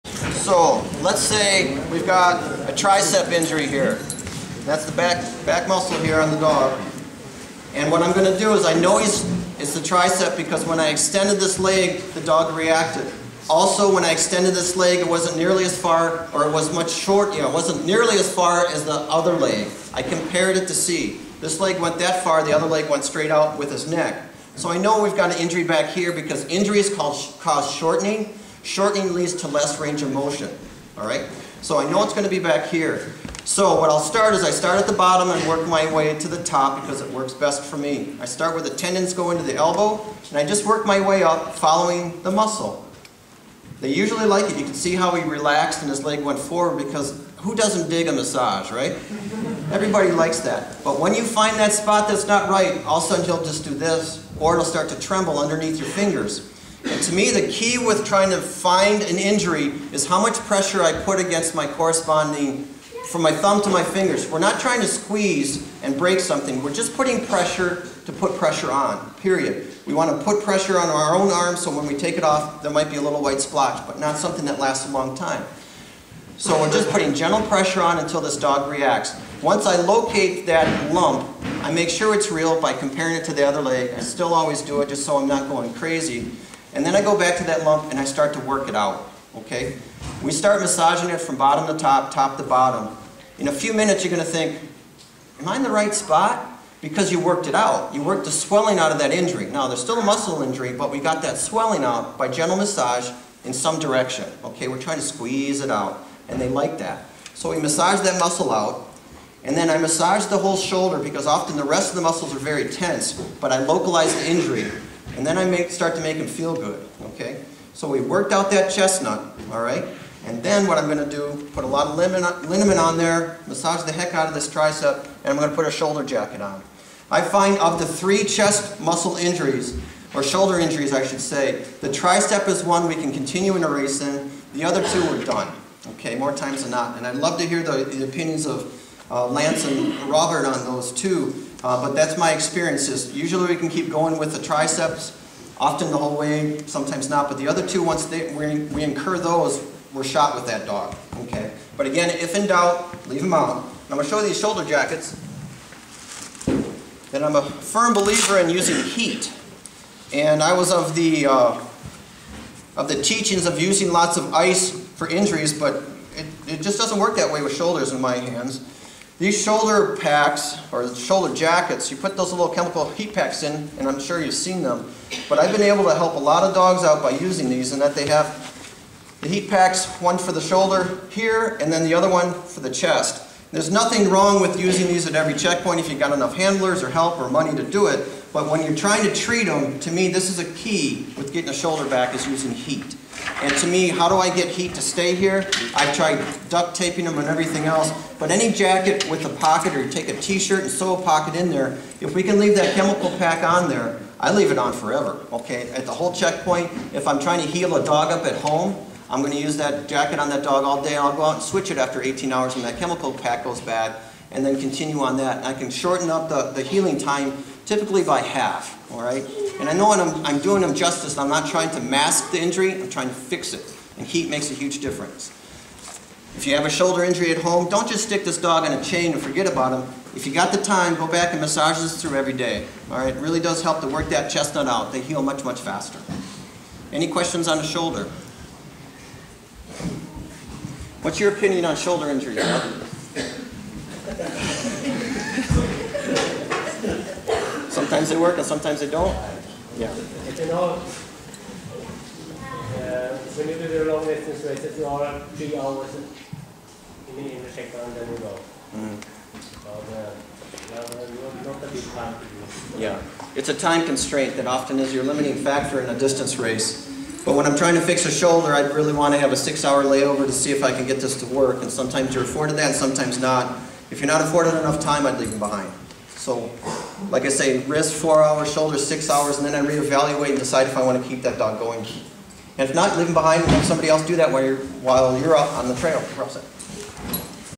Sled dog examination shoulder injure by Tim Hunt
Veterinarian and dog musher Tim Hunt explains how to treat a shoulde injure  to a sled dog. Sled Dog seminar in Harestua Norway 2010.